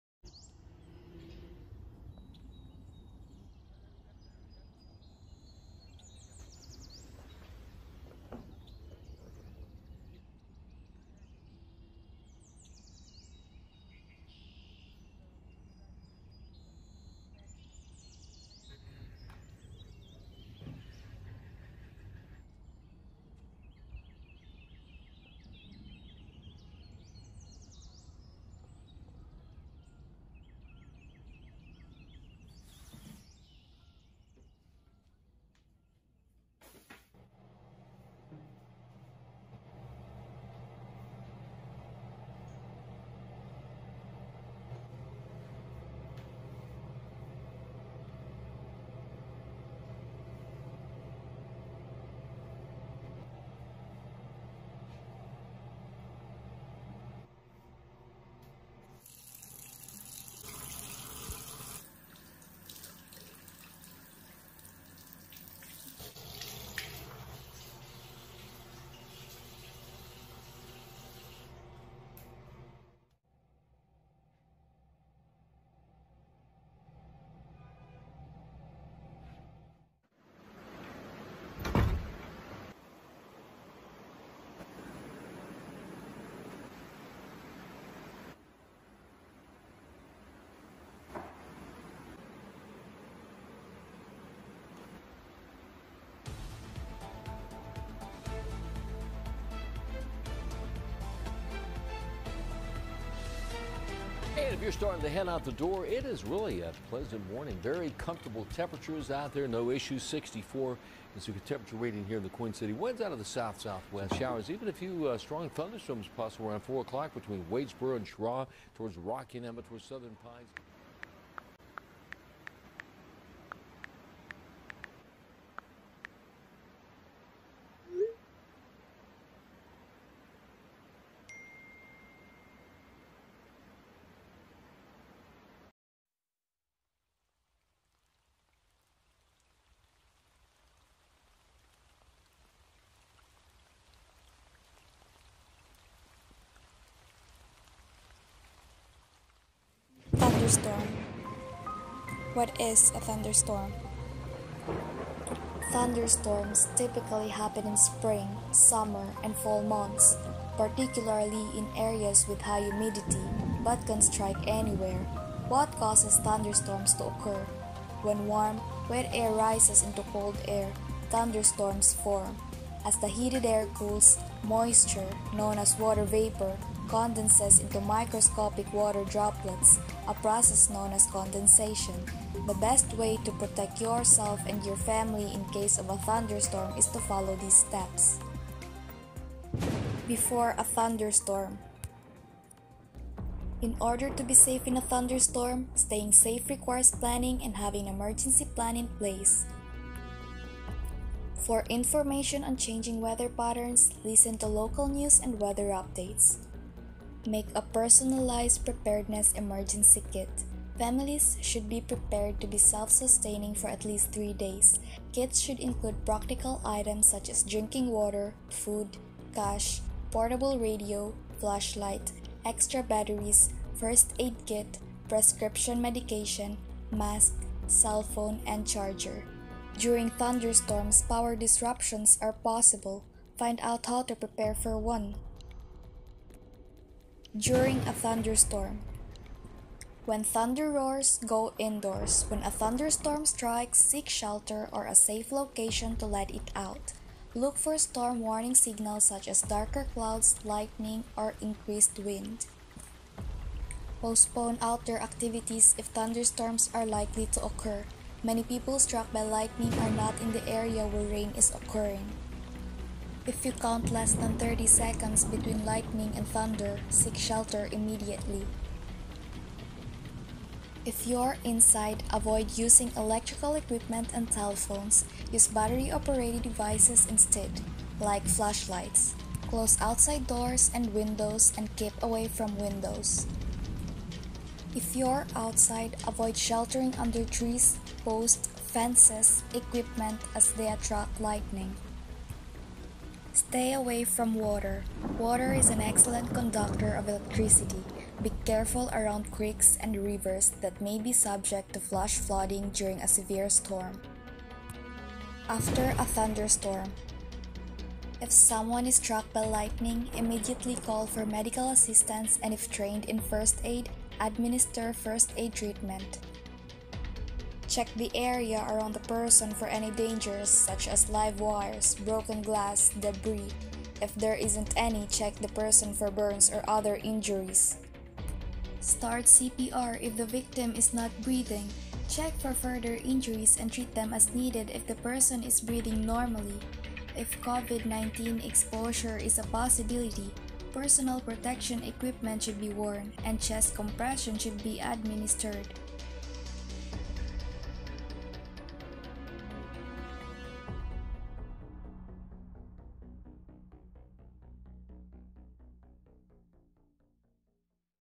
Thunderstorm | DRRR | 11-1
GSDMSFI | DRRR-1 | Hydrometeorological Hazards

Leader: Rose Dipus
Editor: Angel Palmes

Starring:
Jocelyn Yu
Chelsea Sumalpong
Aylin Ates
Samantha Rodas
Marc Condino
Thea Lumanta